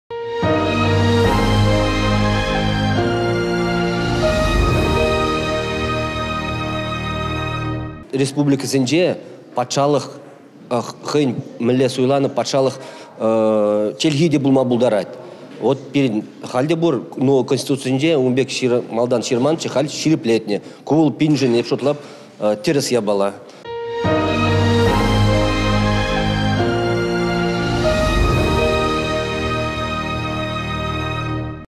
Раҫҫей Федерацийĕн Конституцине кĕртекен улшӑнусене Сергей Павлов хаклать
Раҫҫей Федерацийĕн Конституцине кĕртекен улшӑнусене Чӑваш Республикин Патшалӑх Канашĕн депутачĕ Сергей Павлов хаклать